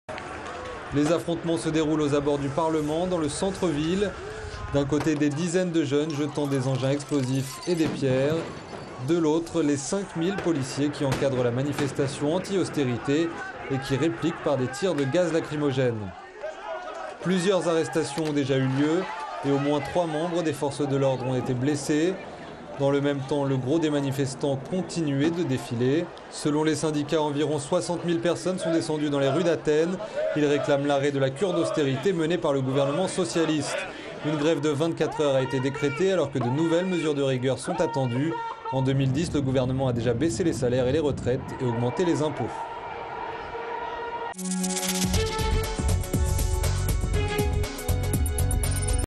Greece riots and clases with molotof in Athens 23 Feb.
See more on gooclip.net
23 Februarry strike in Athens, riots and clases against police with molotof coctails, Manifestants and dog anarchist against police. War situation in Athens and near the parliament